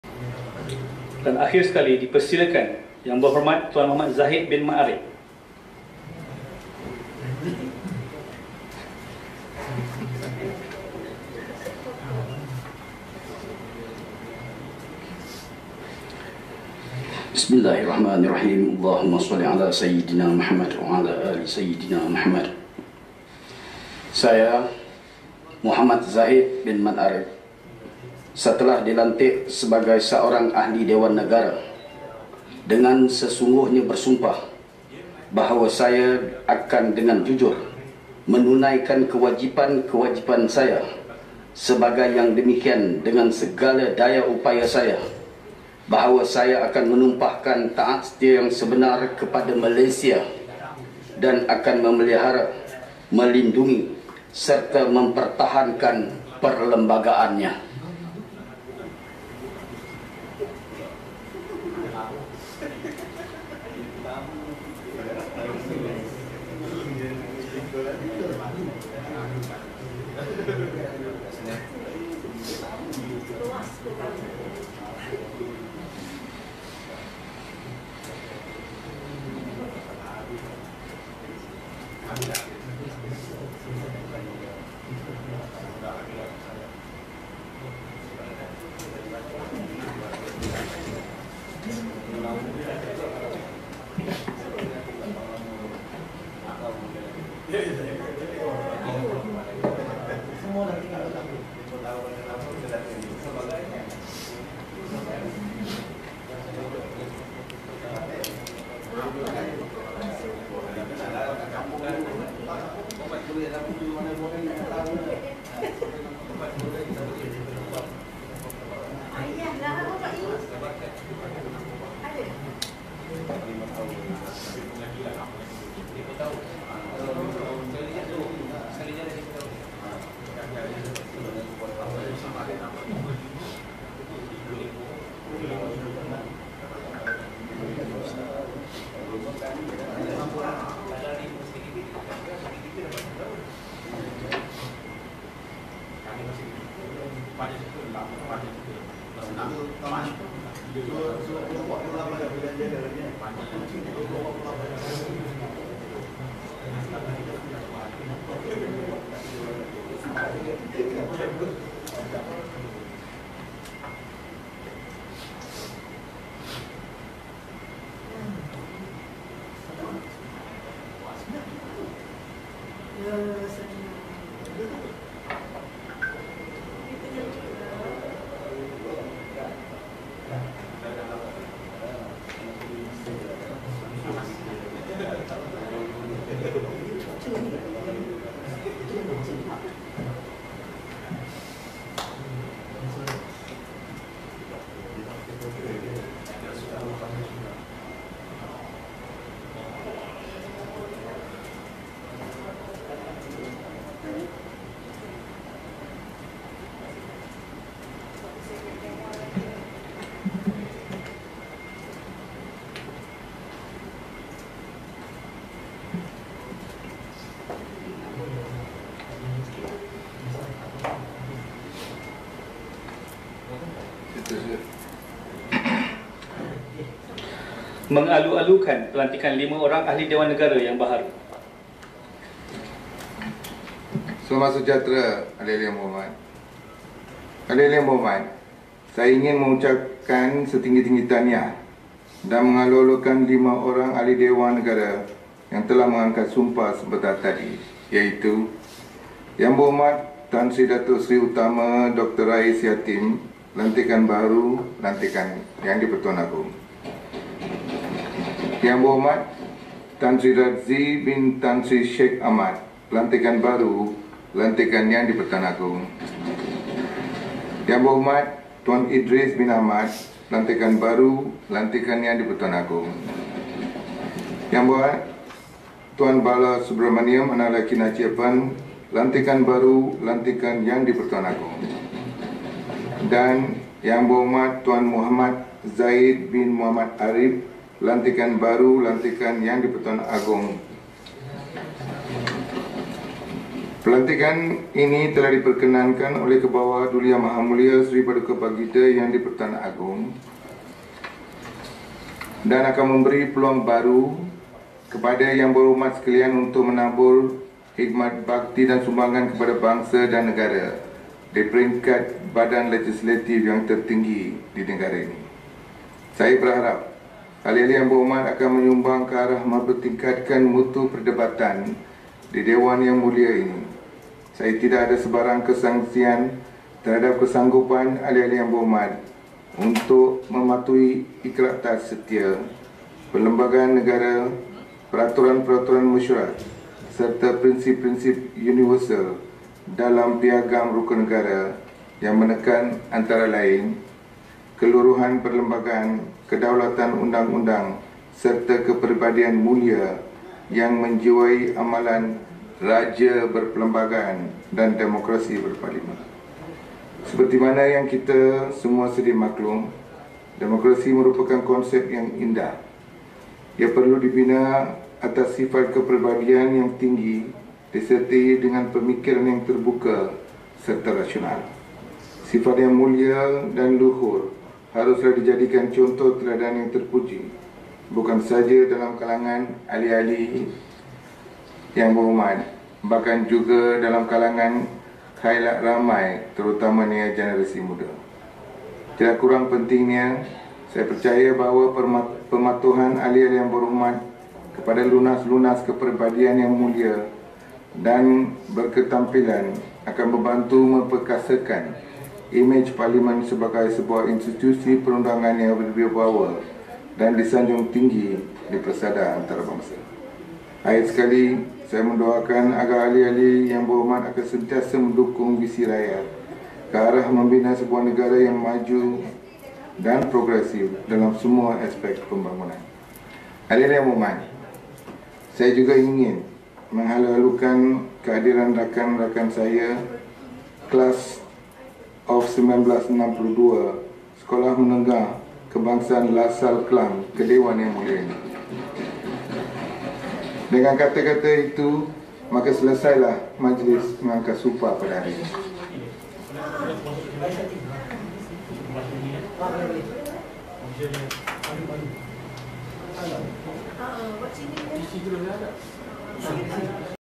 20200616 Zahid Md Arip angkat sumpah sebagai Senator
Empat lagi Senator baru yang mengangkat sumpah hari ini ialah

1. Tan Sri Dr Rais Yatim - PPBM
2. Tan Sri Sheikh Radzi Sheikh Ahmad - PPBM
3. Naib Presiden Pas Idris Ahmad - PAS
4. N Balasubramaniam, Ketua Dewan Penyokong Pas (DHPP)